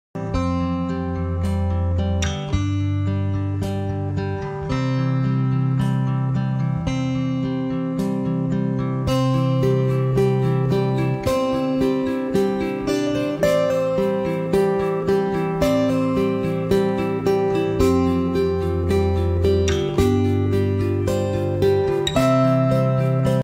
Largest Butterfly In The World. To know more link in comments
Queen Alexandra's Birdwing butterflies are the largest species of butterfly on earth.